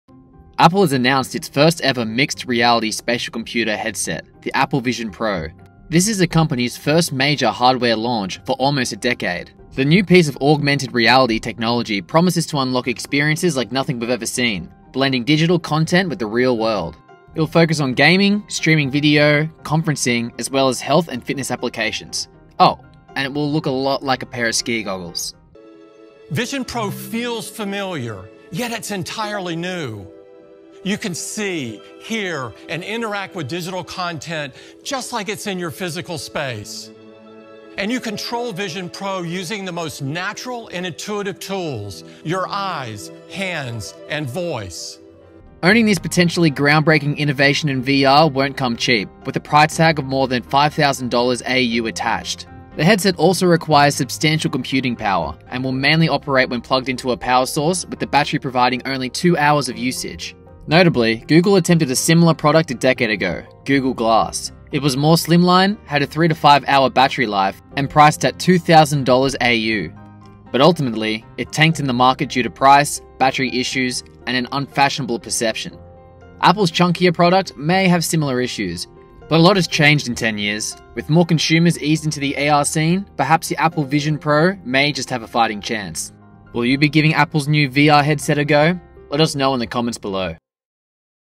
Apple announces its first VR headset
Apple has launched its first-ever mixed reality “spatial computer” headset, the Apple Vision Pro – but it has a hefty price tag.